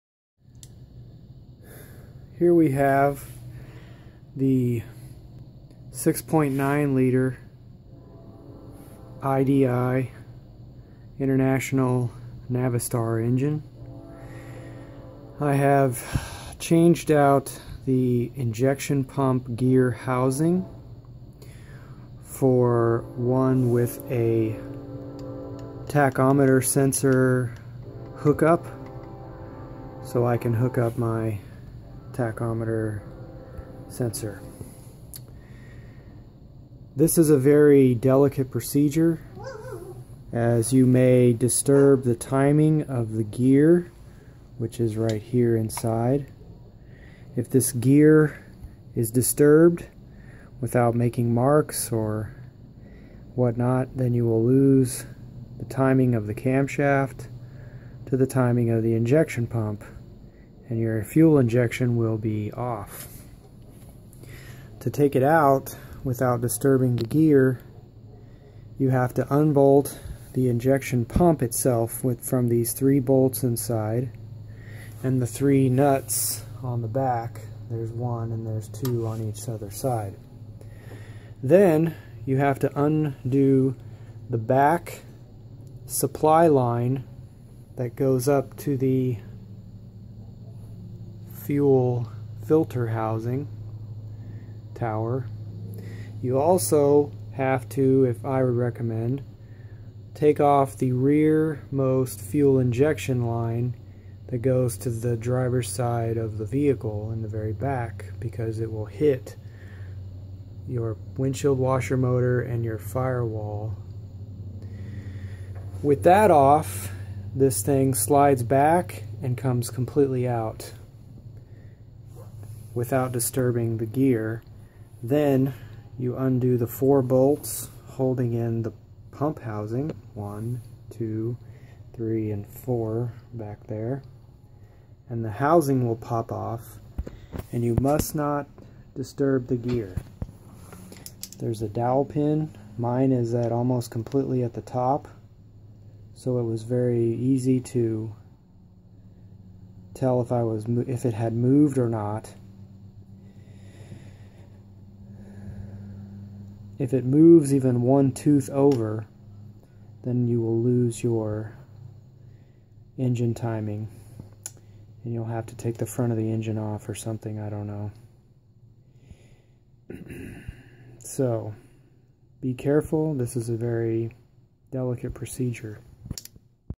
6.9/7.3 idi diesel timing gear housing removal.
Remove the I.P. and timing gear housing without disturbing the timing gear.